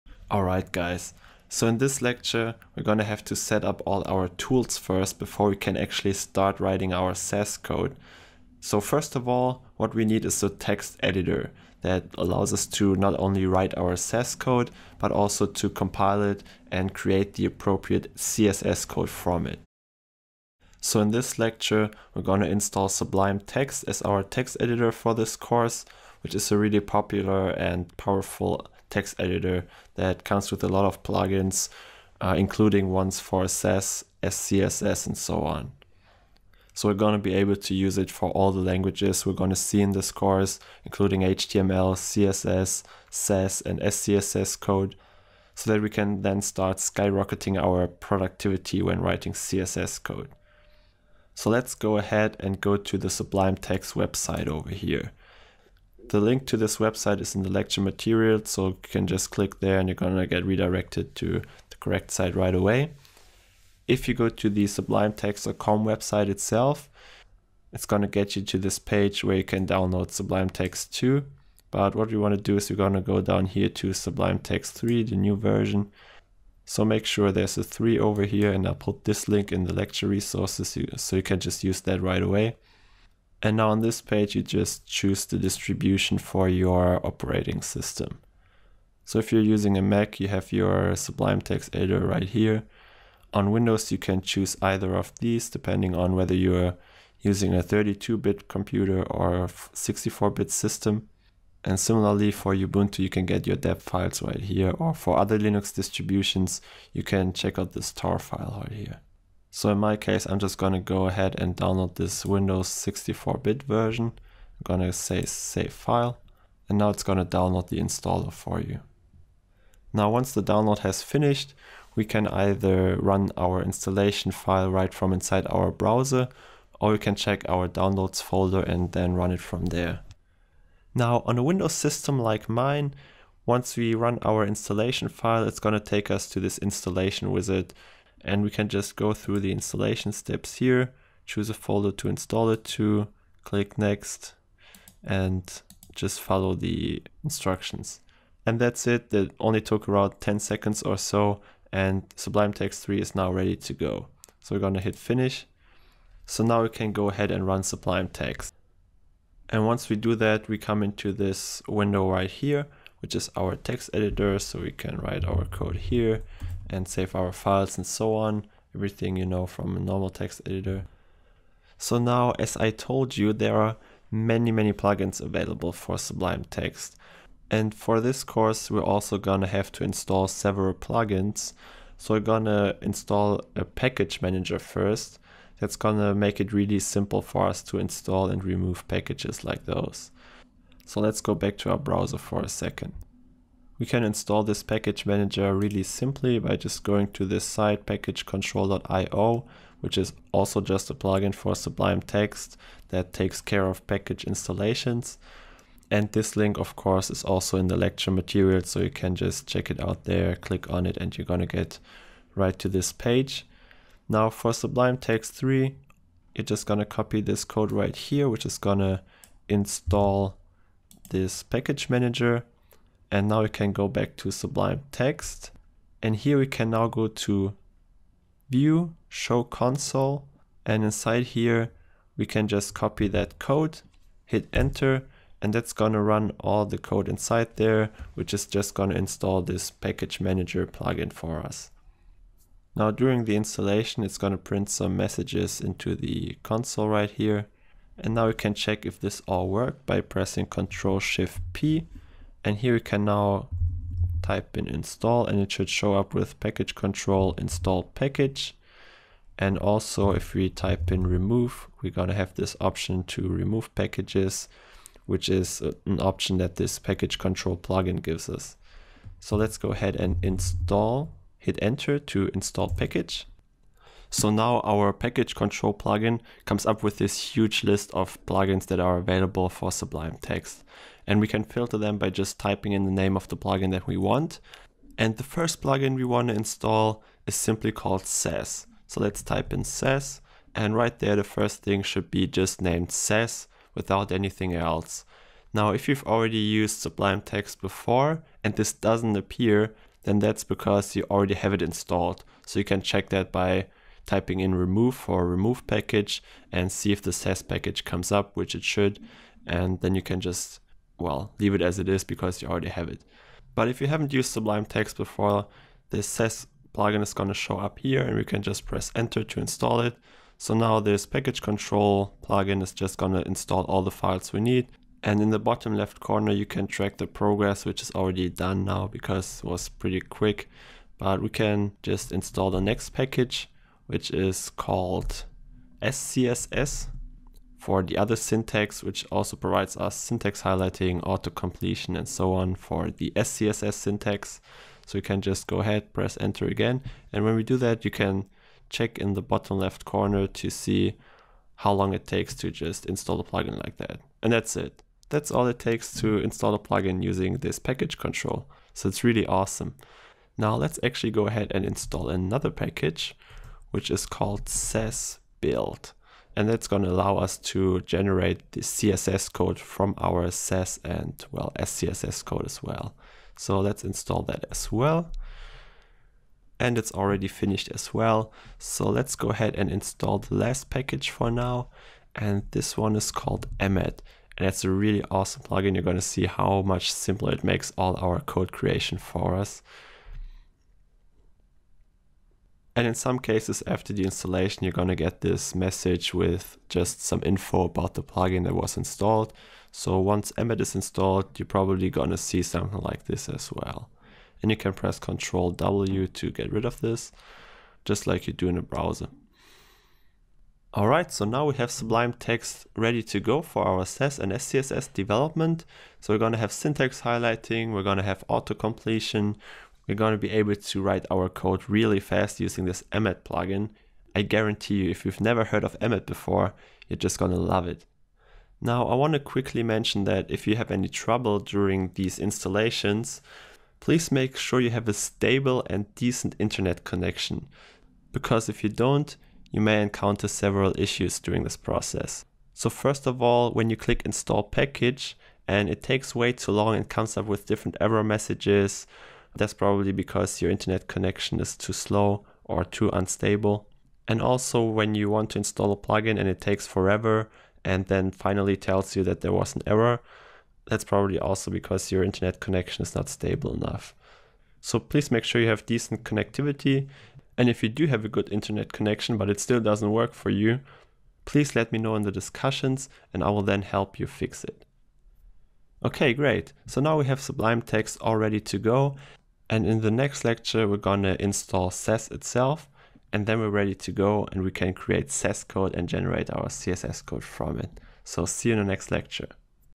Sass Beginner Tutorial #3: Setting Up Sublime Text 3
COMING IN NOV 2020: Join my free 6-part video series to become a confident software developer

Sublime Text is a very popular, lightweight editor that you can use very well for things like Sass. This quick guide shows you how to set it up correctly to write and build Sass code.

There are quite a few steps involved so I recommend you follow along closely to be able to really dig into Sass and SCSS.

Let me know what other topics you want to learn about (takes just 1 minute)